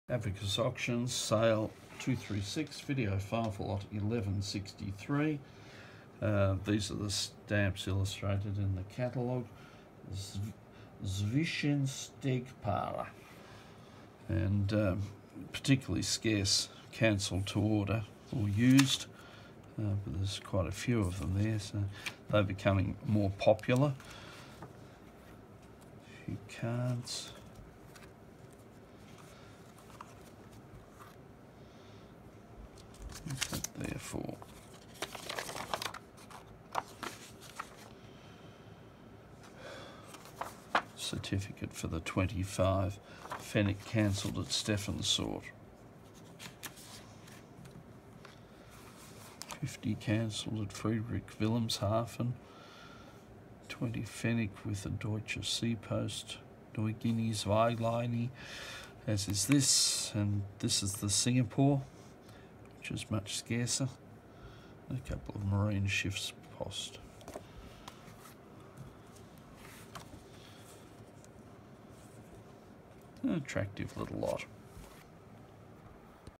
Auction 236 Lot 1163
Neat collection with Vorlaufer 25pf with superb 'STEPHANSORT' cds (Cat €550: with Certificate) & 50pf with 'FRIEDRICH-WILHELMSHAFEN' cds, Overprints used set with shades of 25pf & 50pf plus Zwischenstegpaare low denomination gutter pairs & blocks of 4 (**), three listed varieties & 40pf Yacht with minor variety, a few postmarks including very fine '.../NEU-GUINIE/ZWEIGLINIE/(SINGAPORE)' cds, and four 10pf Postal Cards CTO x2 or postally used to Germany or Switzerland, generally very fine. (Qty 3)